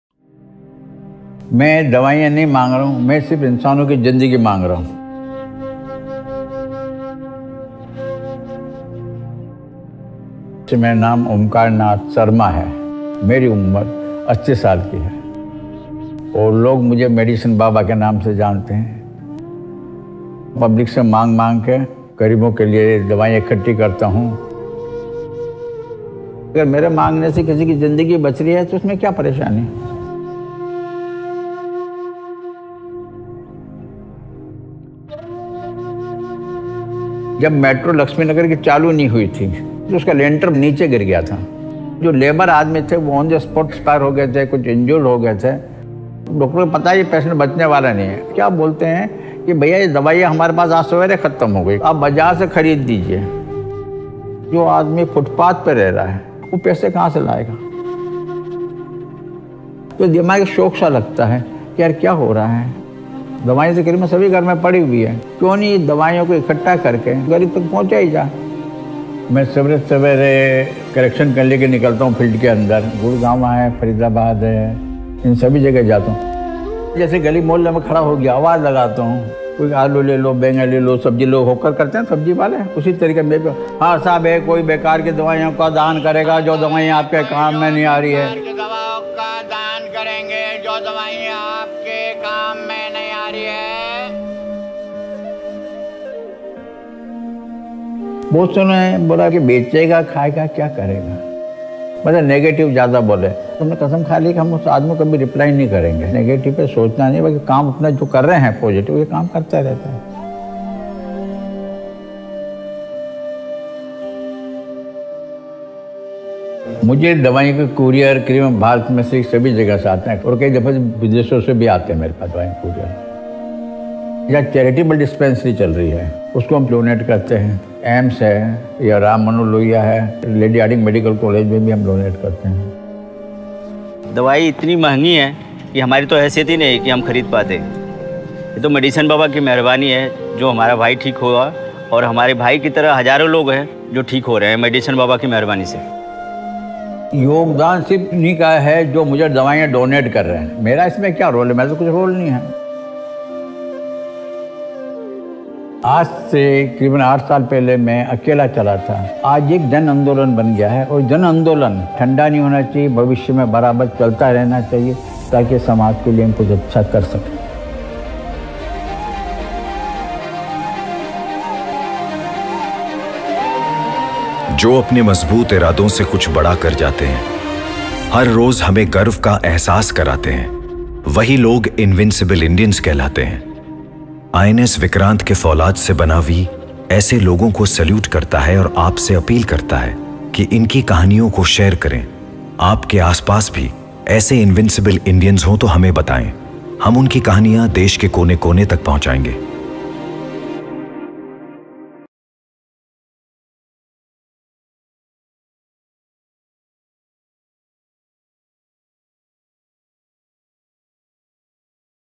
Cashlees Free medicine by Medicine Baba । Most Viral Video । समाज सेवक बाबा
A 80 year old man works in Delhi for poor people. He begged medicine from every person for helping of poor people. This is very big step for social work.. Please see this video and share more and more.